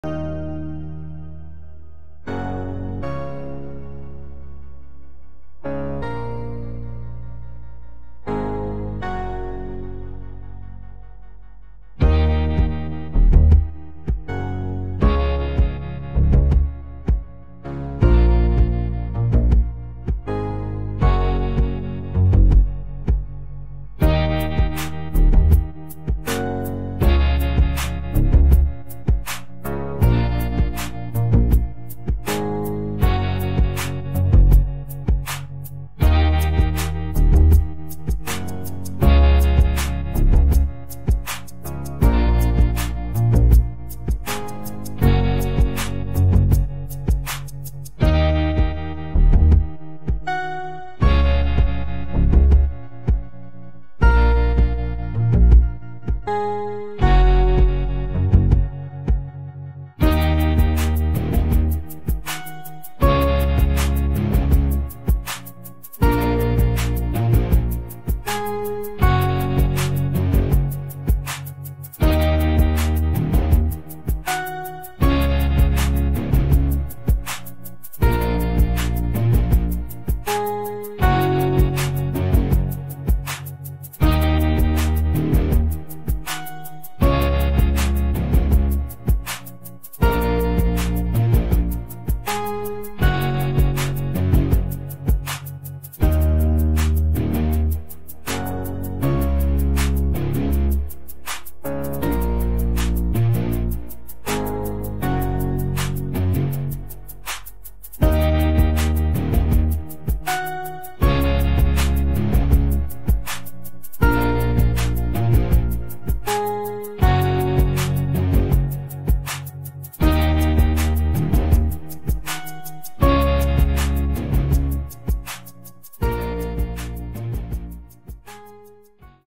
Homemade birdhouses
Two homemade birdhouses made for our moms on mothers day 2020. Music credit runrunricky